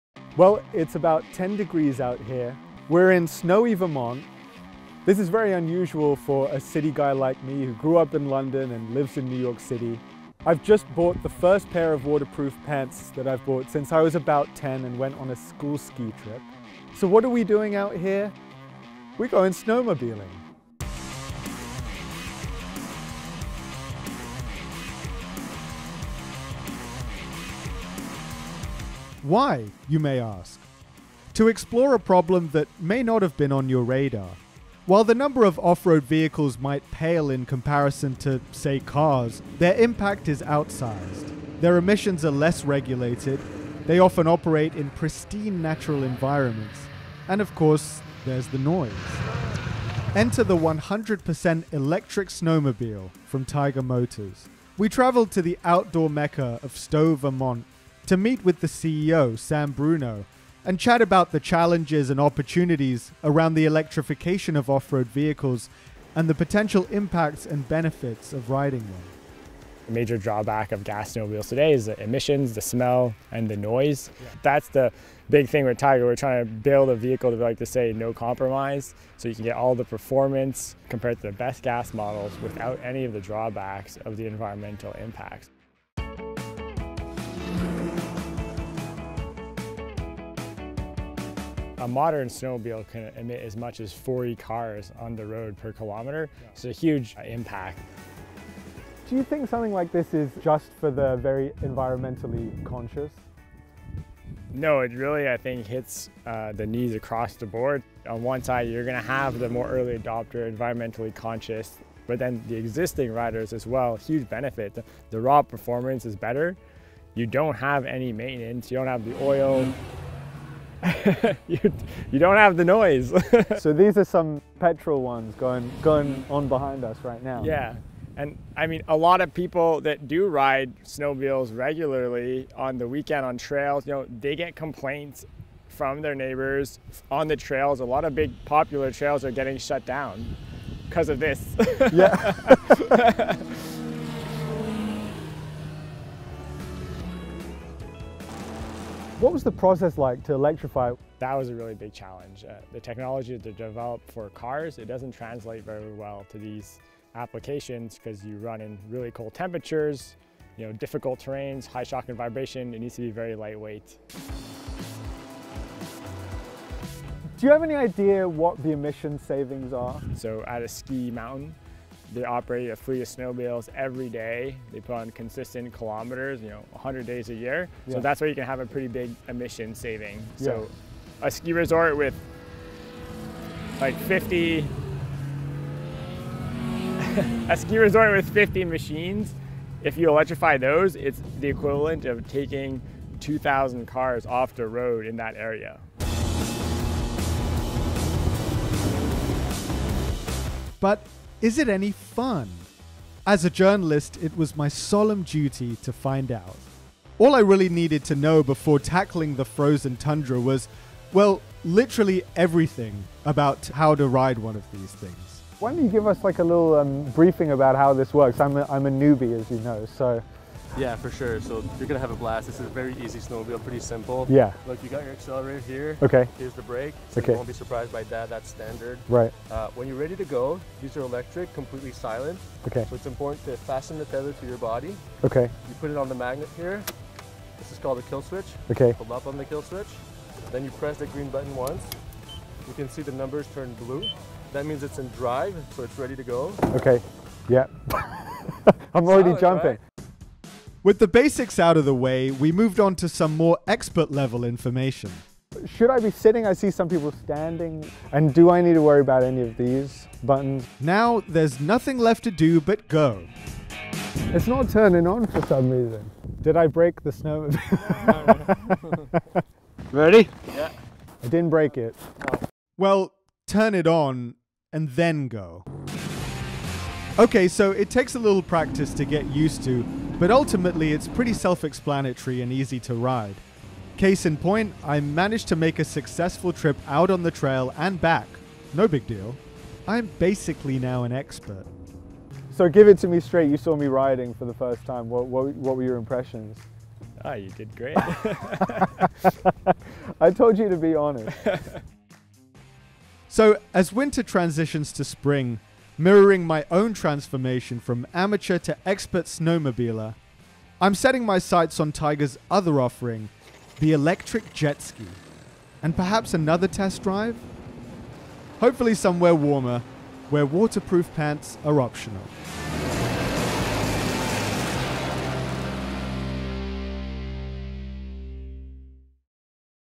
First Production Electric Snowmobile From Taiga Could Have an Outsize Impact| Fast Company
Can the first-production electric snowmobile from Taiga Motors really make a meaningful impact on the environment? Sure, the number of off-road vehicles in the world might pale in comparison to, say, cars, but with their emissions less regulated, they often operate in pristine natural environments, and then there’s the noise.

Enter the 100% electric snowmobile from manufacturer Taiga Motors. We traveled to Stowe, Vermont, to meet with Taiga CEO Sam Bruneau about the challenges and opportunities around the electrification of off-road vehicles, and the potential impacts and benefits of riding the ‘Tesla of Snowmobiles.’